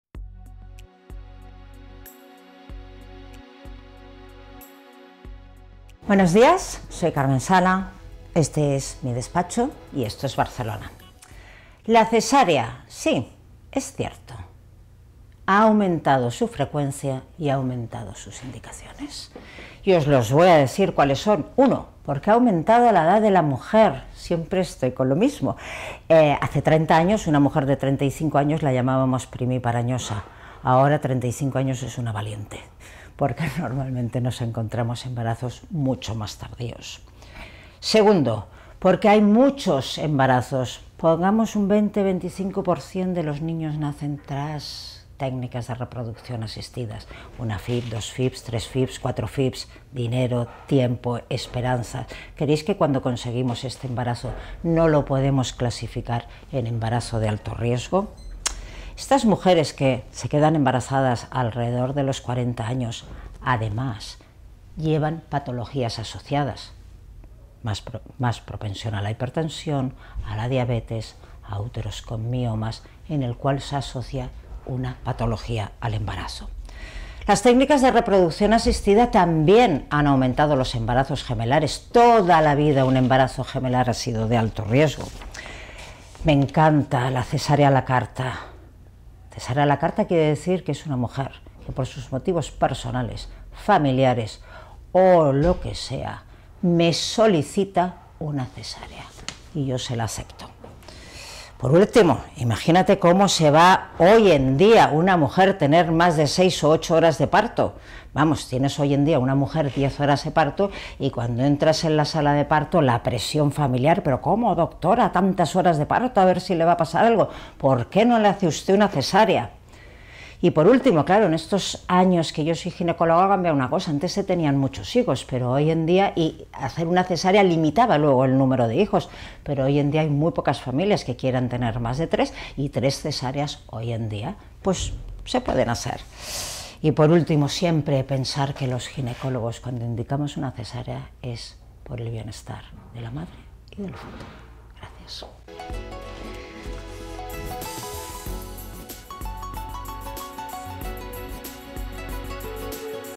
Cesárea: ¿una opción médica para nacer o una demanda social?
Barcelona, 05 jul (efesalud.com). Para la  doctora Carmen Sala Salmerón, ginecóloga y obstetra de la Clínica Gine-3 de Barcelona la respuesta es clara y contundente: "Cuando indicamos un parto por cesárea, sea la razón fundamental un problema médico previo o sobrevenido en el paritorio, o por una circunstancia vivencial de la madre, nuestro pensamiento busca exclusivamente el bienestar de la mujer y el feto; aunque no es fácil decidir lo que es mejor para ambos".

¿Y entonces, por qué han aumentado tanto el número como la frecuencia de las cesáreas?

"Son varias las causas: cada vez más mujeres se quedan embarazadas a edades tardías y, además, pueden presentar patologías asociadas como hipertensión, diabetes o útero con miomas; gestaciones inducidas a través de técnicas de reproducción asistida, que a su vez propician embarazos gemelares; cesáreas voluntarias a petición de la madre o impulsadas por el estrés familiar a las puertas del paritorio; y, finalmente, el actual bajo número de hij@s por unidad familiar", relaciona.